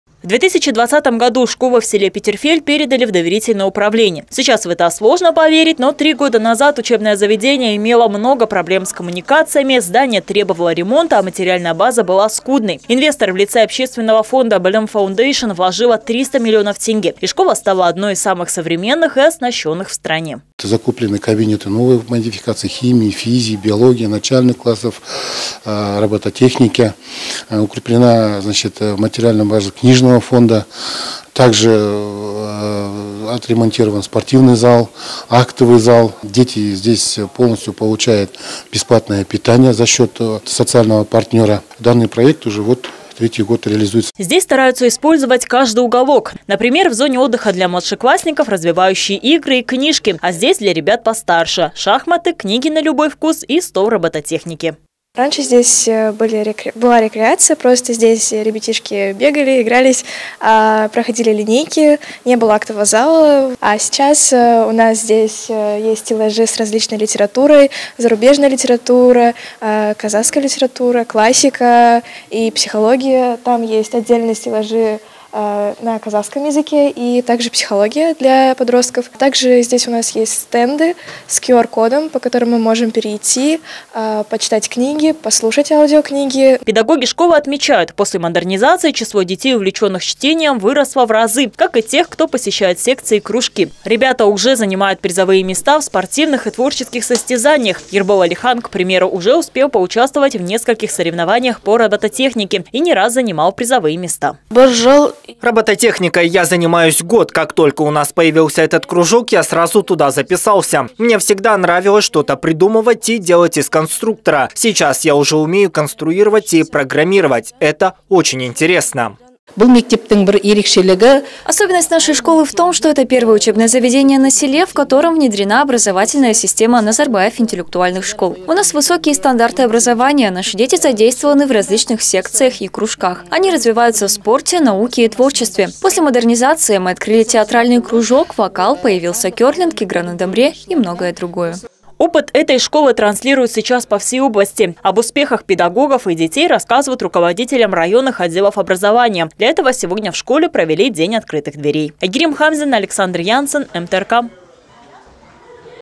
В школе с.Петерфельд делятся успешным опытом ГЧП
В 2020 году школу в селе Петерфельд передали в доверительное управление. Сейчас в это сложно поверить, но 3 года учебное заведение имело много проблем с коммуникациями, здание требовало ремонта, а материальная база была скудной. Инвестор в лице ОФ «Bilim Foundation» вложило 300 млн тенге и школа стала одной самых современных и оснащенных в стране.